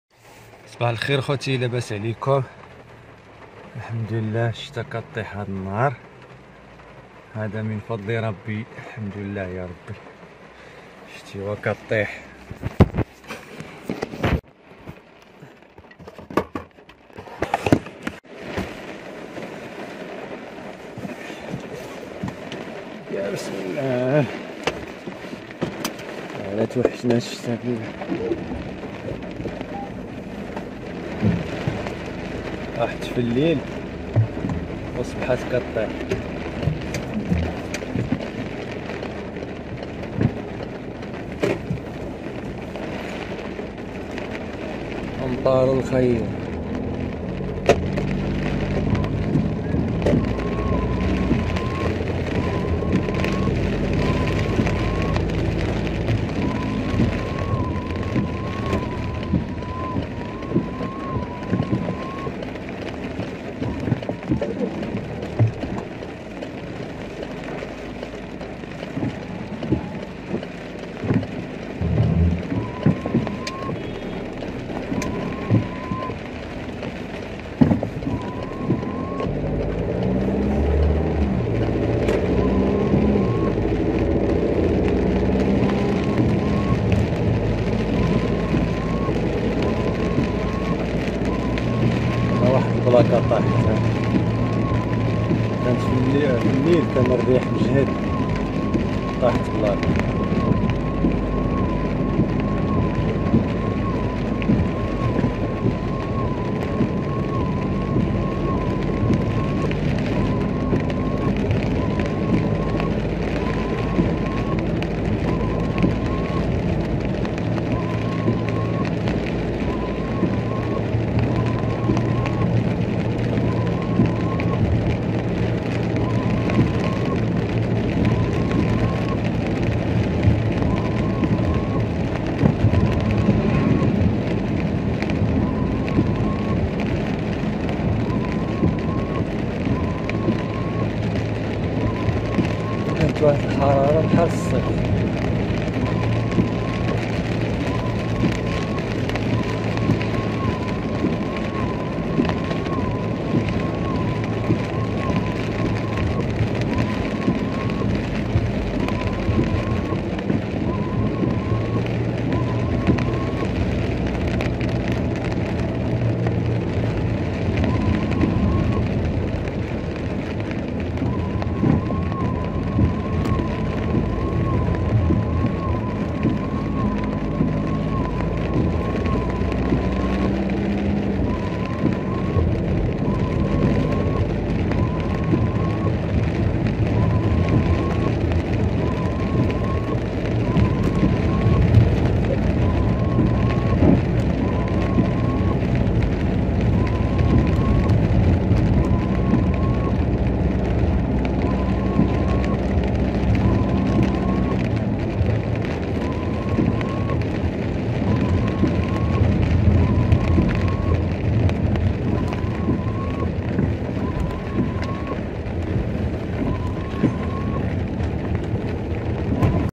أمطار الخير تروي أرض مكناس
أمطار الخير تروي أرض مكناس، تحمل الحياة والزهور، تبث البهجة وتنعش الطبيعة بلمسة رحمة سماوية